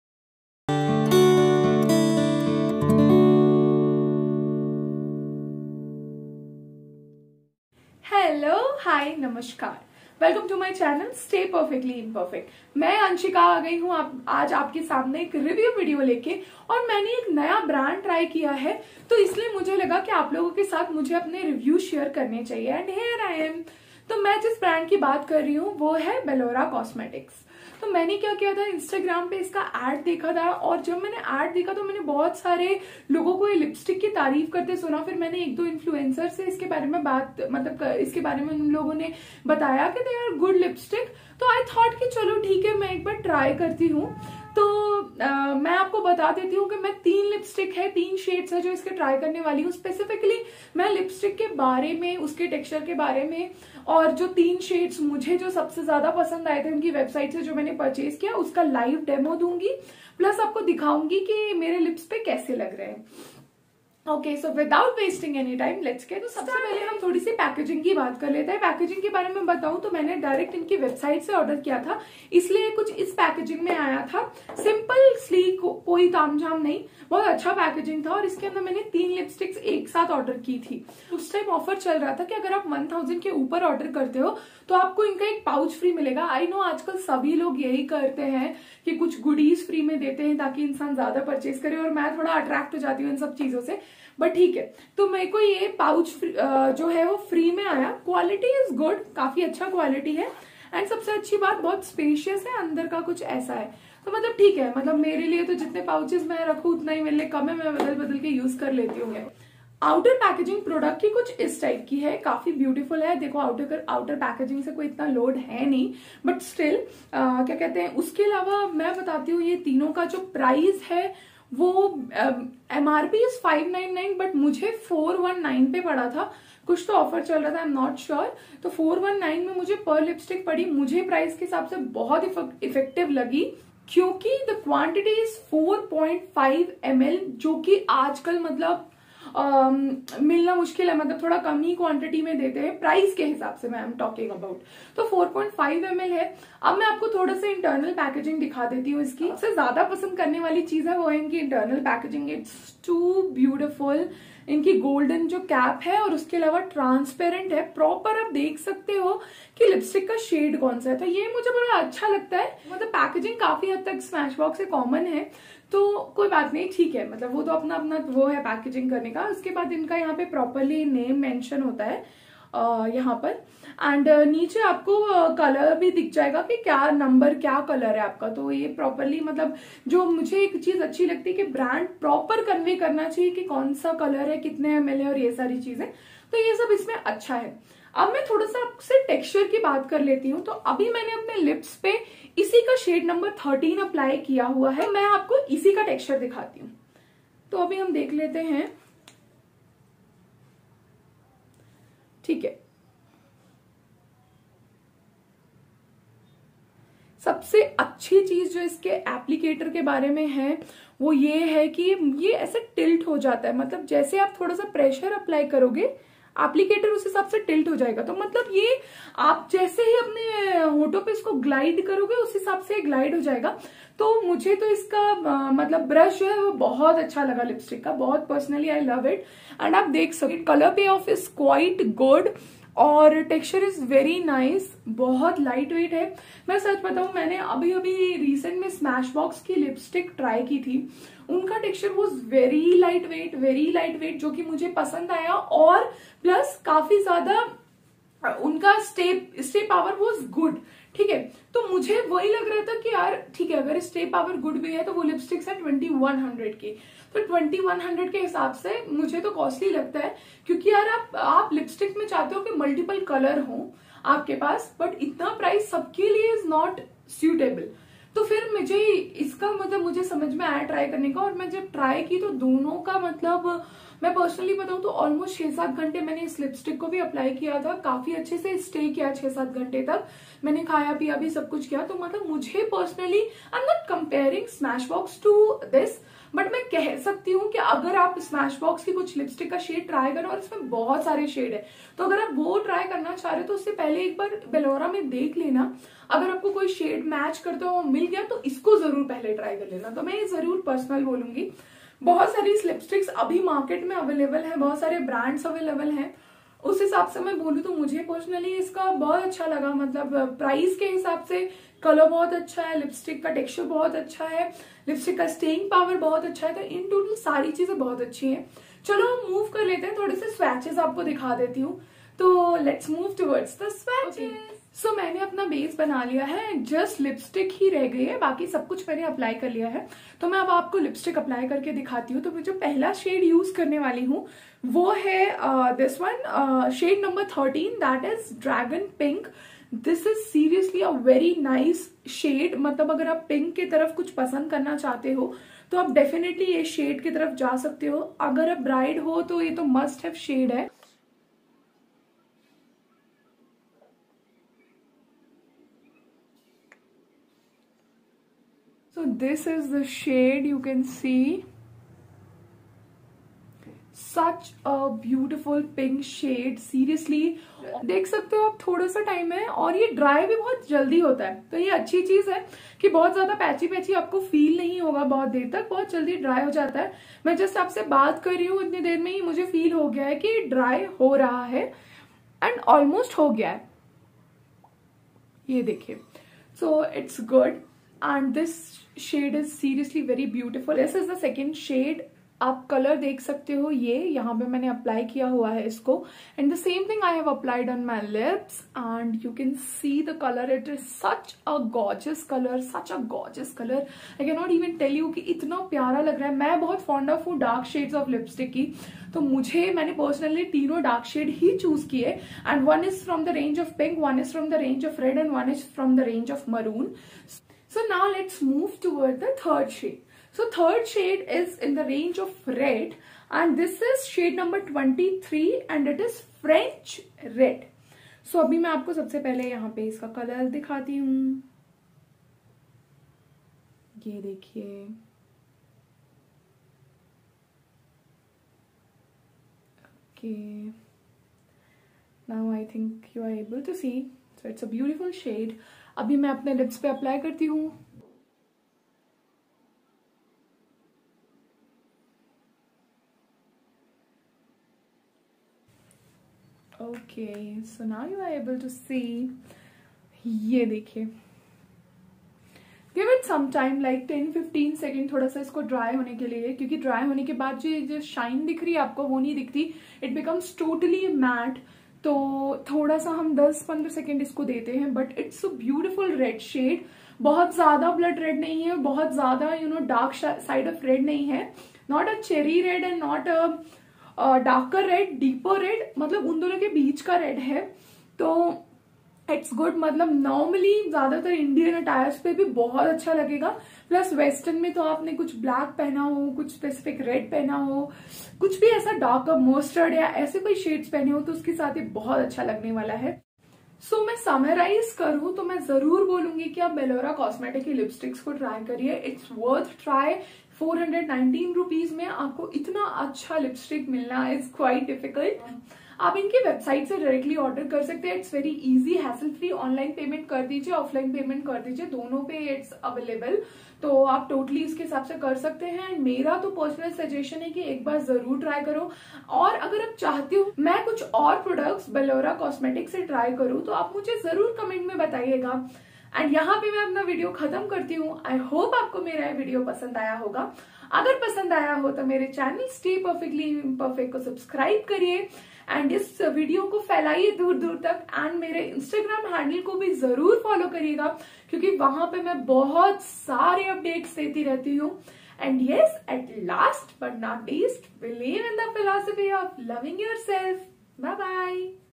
BELORA COSMETICS No Evidence Liquid Lipstick Review & Swatches|Belora Paris|Stay Perfectly Imperfect
Hello Hi Namaskar,

Welcome Back...!
In todays video I have shared my review + Demo of Liquid Lipsticks from the brand "Belora Cosmetics".

Below are the details:
Quantity  : 4.5ml
Price : Rs. 599/- each
Shades Available in the Range: 12 Shades

Shades Swatched :
* Dragon's Pink---shade no 13
* Maroon Grapes---shade no 18
* French Red--- shade no 23